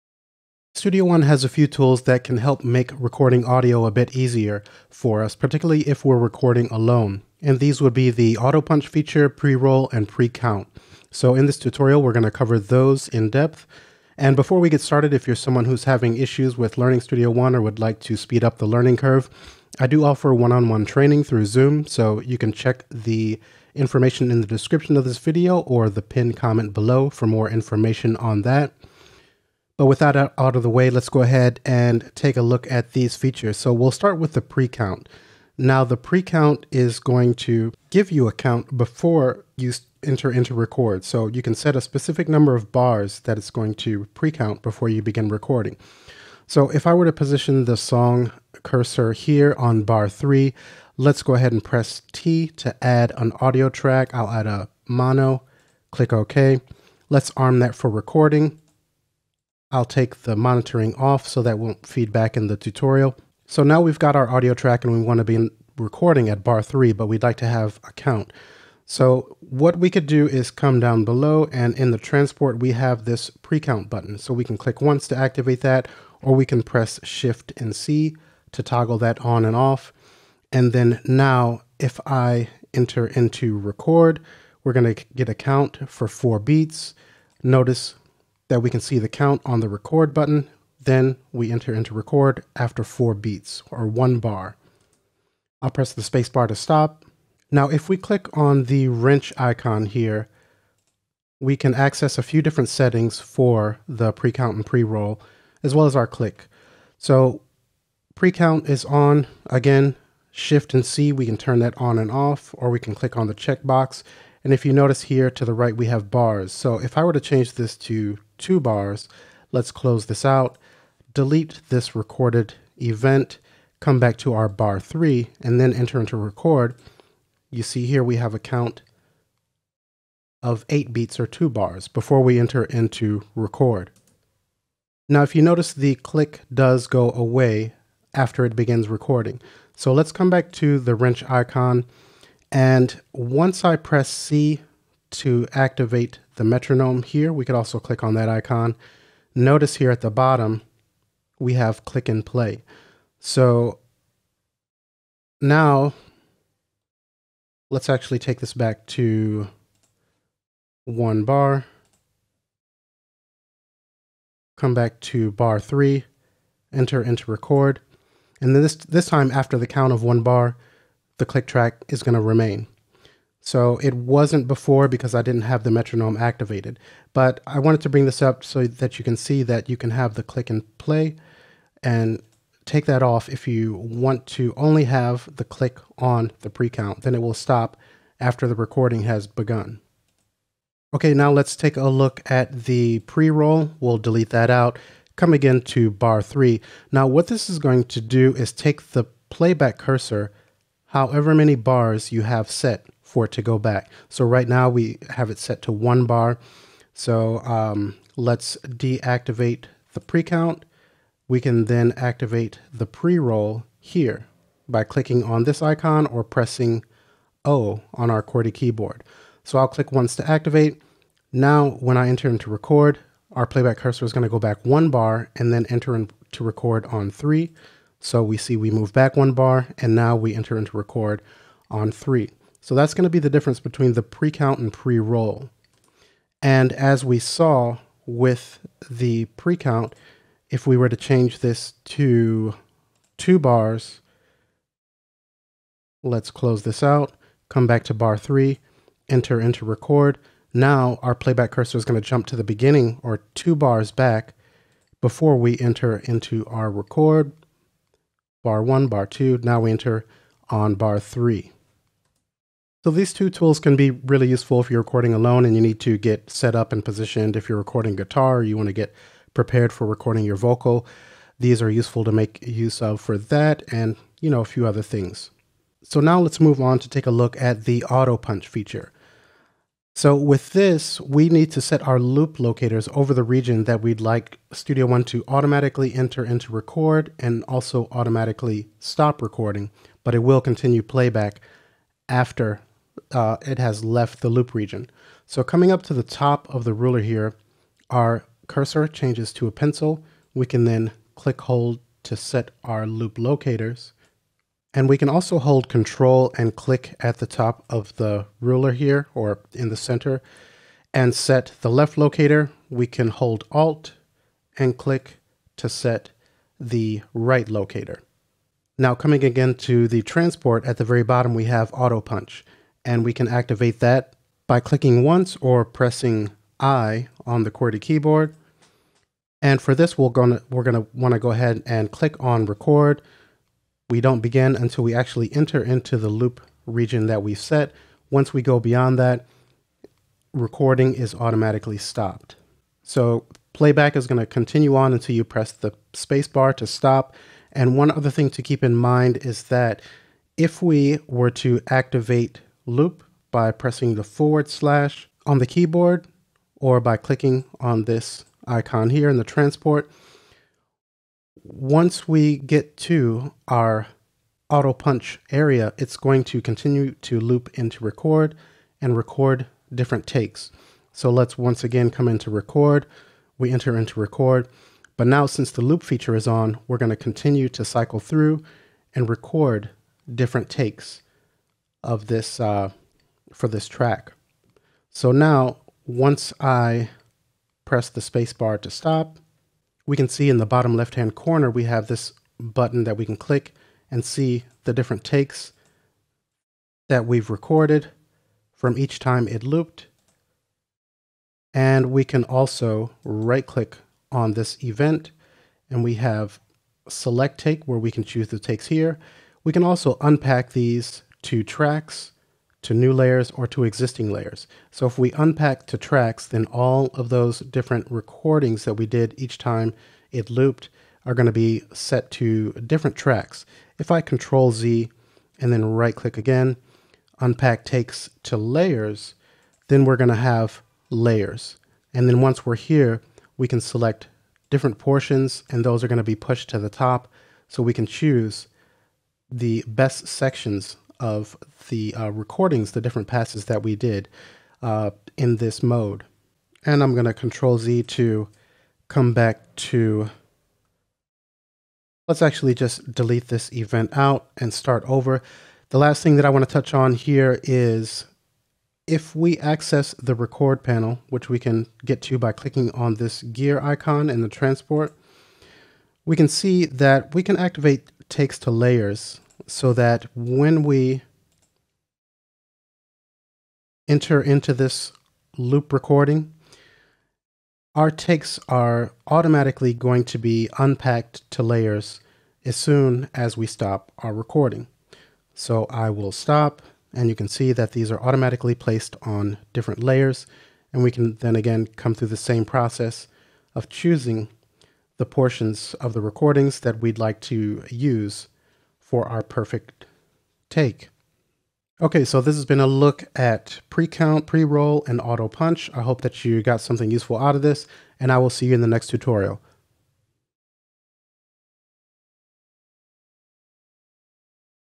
Learn Studio One 5.4 | Using Auto-Punch, Pre-Count, & Pre-Roll
In this tutorial we take a look at how to use the Auto-Punch, Pre-Count, & Pre-Roll functions within Studio One 5.4.